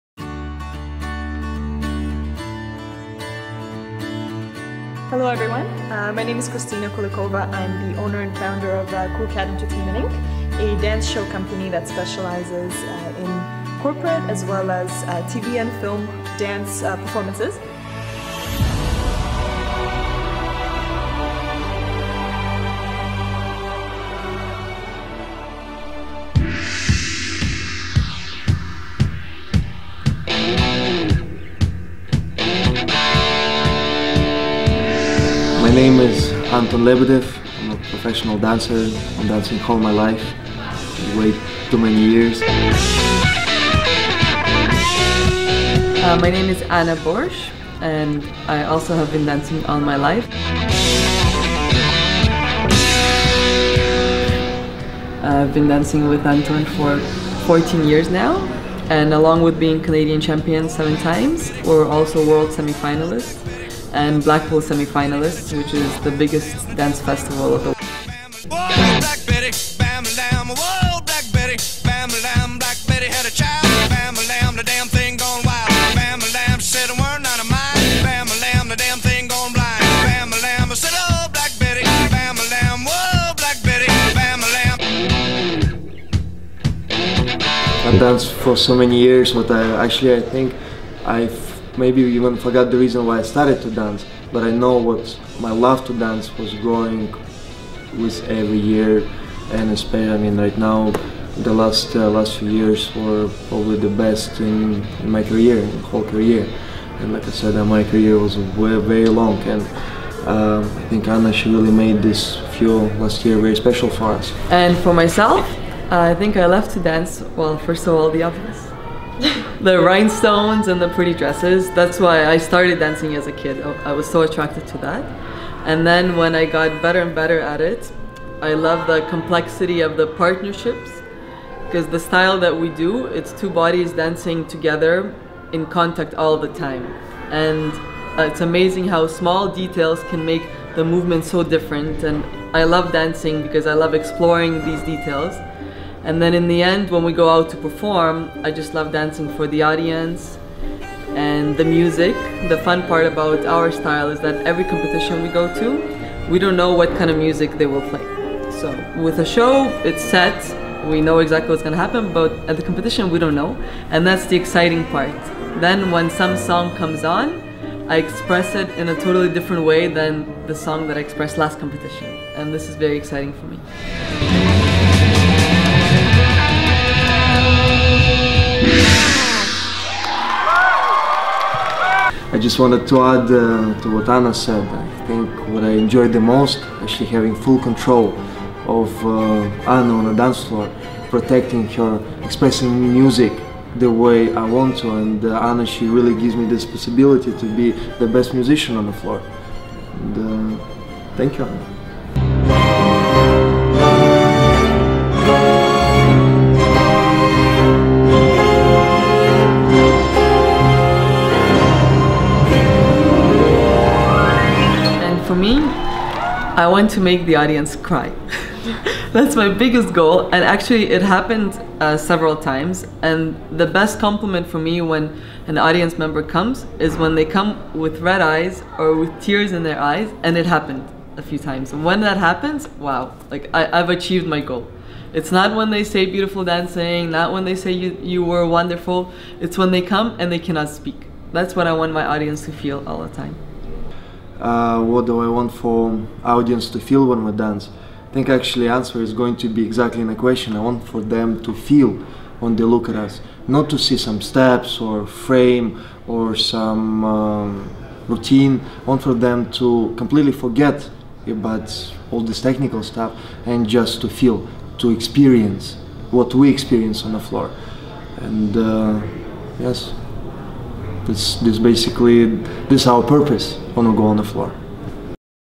Dancer's Passion Series- Ballroom Champions Ep.-Ft. Anton Lebedev & Anna Borshch-KulKat Inc.
KulKat Entertainment is honored to have a chance to work with one of the most highly accomplished Ballroom couples in the world: Anna Borshch & Anton Lebedev who are world renowned dancers & performers, as well as talented instructors and coaches of their craft!
In this episode of Dancer's Passion Series- meet Anton & Anna, learn about their inspiration behind the dancing, and of course see their action packed & dramatic show numbers!

Dancer: Anton Lebedev & Anna Borshch
Videographer: LightGlam-Antonio Caputo
Filming Location: Dance Mania Dance Studio, Richmond Hill
Producers: Kristina Kulikova of KulKat Entertainment, Antonio Caputo of LightGlam